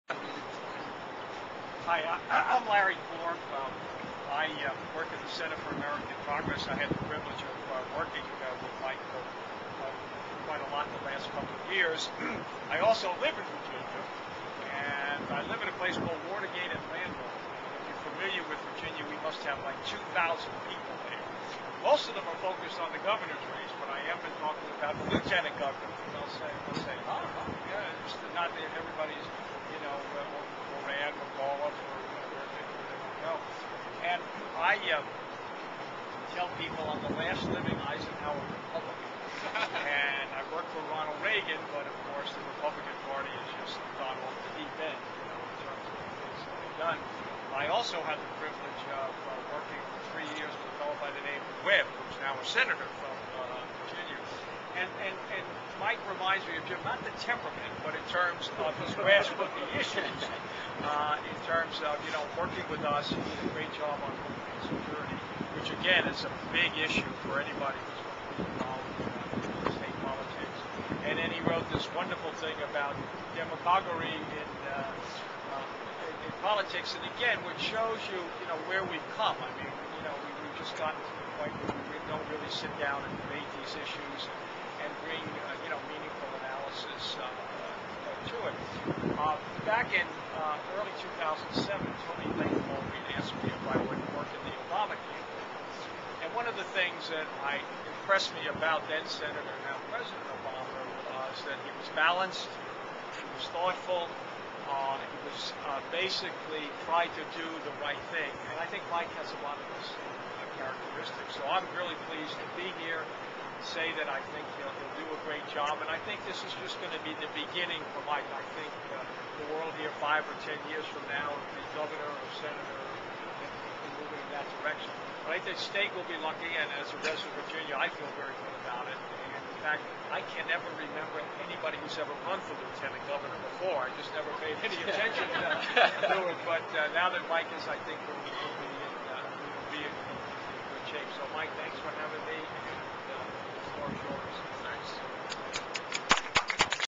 Larry Korb for Mike Signer
Larry Korb of the Center for American Prorgress speaks at an event for Virginia Democratic Lieutenant Governor candidate Mike Signer on June 1, 2009. The even twas held on the rooftop of a high-rise on Connecticut Avenue in Washington, DC. The election is Tuesday, June 9.